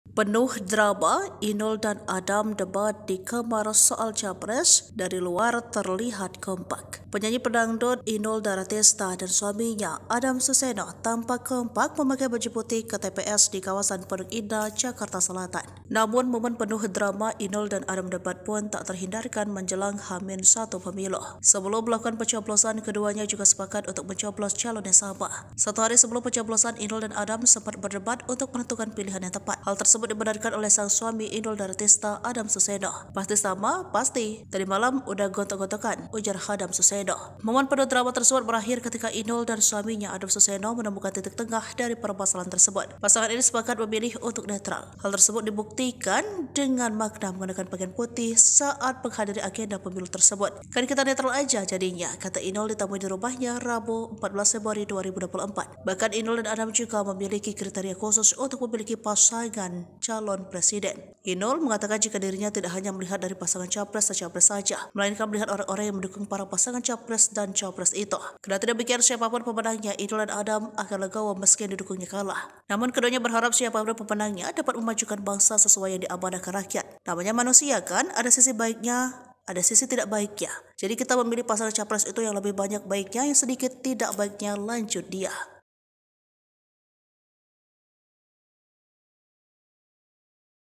Penuh Drama, Inul dan Adam Debat di Kamar Soal Capres, dari Luar Terlihat Kompak
Penyanyi dangdut Inul Daratista dan suaminya, Adam Suseno tampak kompak memakai baju putih ke TPS di kawasan Pondok Indah, Jakarta Selatan.

Namun, momen penuh drama, Inul dan Adam Debat pun tak terhindarkan menjelang H-1 pemilu

Sebelum melakukan pencoblosan, keduanya juga sepakat untuk mencoblos calon yang sama.

Palembang.tribunnews.com

Sumber Video: localpridegarage
Editor Video: frizeky
Vo: felin